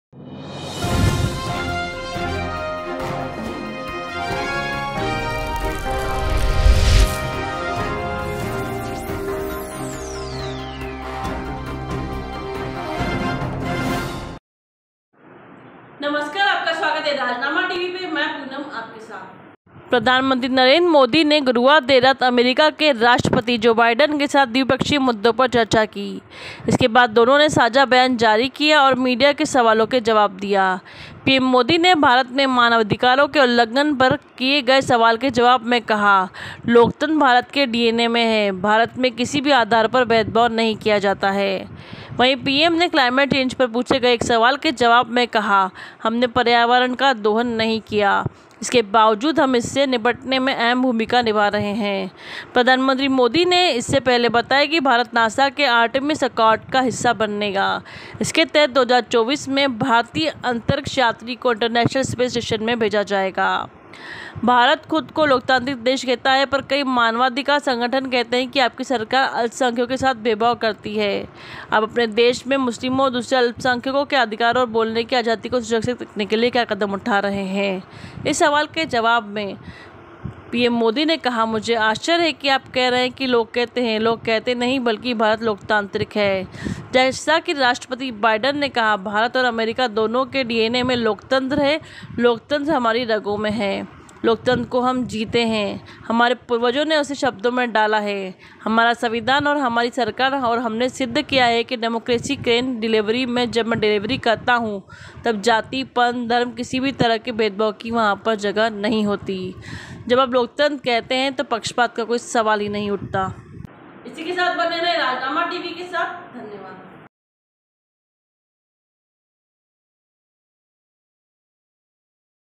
anchorpoonam अमेरिका में PM बोले- लोकतंत्र भारत के DNA में है
About this video:अमेरिका में PM बोले- लोकतंत्र भारत के DNA में है